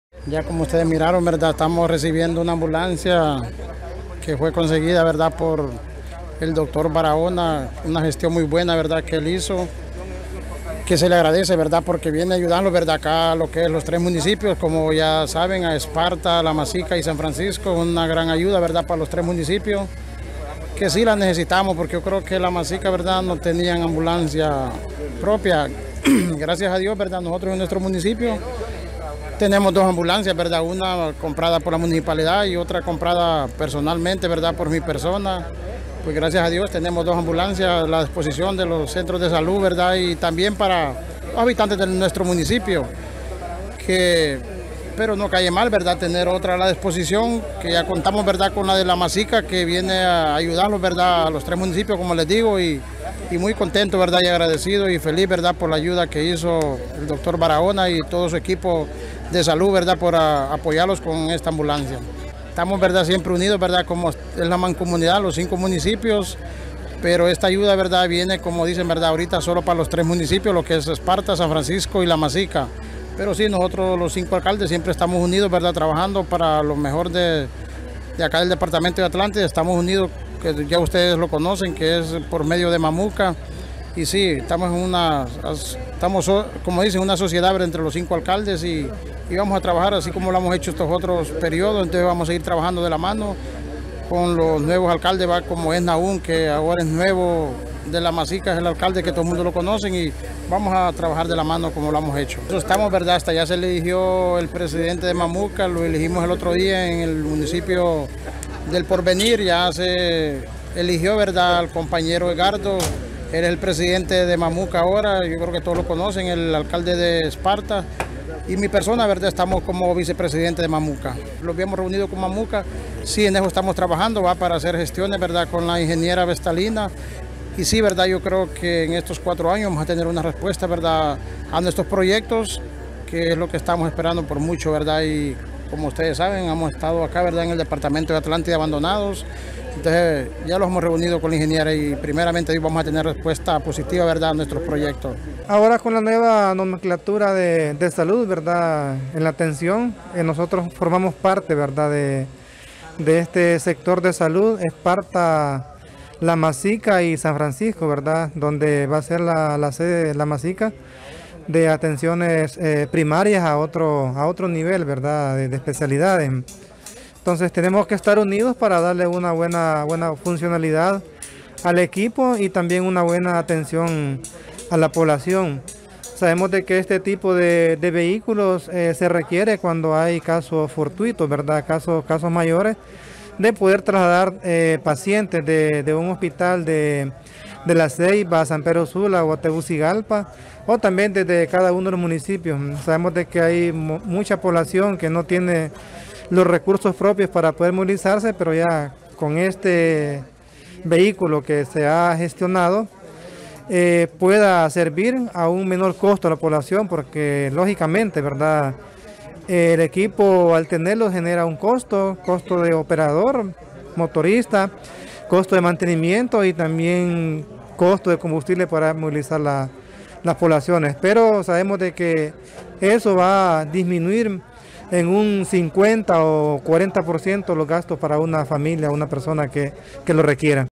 CON LA NUEVA RED DE SALUD, ESPARTA Y SAN FRANCISCO SE VEN BENEFICIADOS POR NUEVA AMBULANCIA
CvaTv Honduras Salud CON LA NUEVA RED DE SALUD, ESPARTA Y SAN FRANCISCO SE VEN BENEFICIADOS POR NUEVA AMBULANCIA

Los alcaldes de SanFrancisco y Esparta, dicen sentirse dichosos por el nuevo vehículo que se donó a la nueva red de salud del centro de Atlántida.

Por su parte los diferentes alcaldes, continúan trabajando juntos en las mancomunidades del centro de Atlántida en desarrollar cada unos de los municipios.